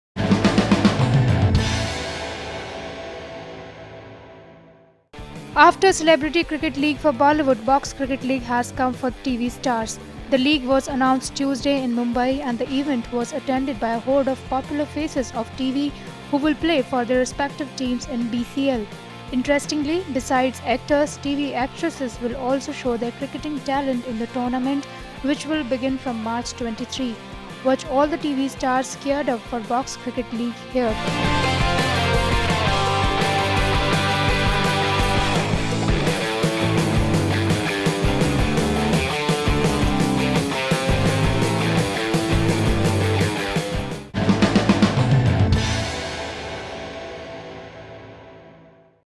TV actors to play cricket now
After Celebrity Cricket League for Bollywood, Box Cricket League has come for the TV stars. The league was announced Tuesday in Mumbai and the event was attended by a horde of popular faces of TV who will play for their respective teams in BCL.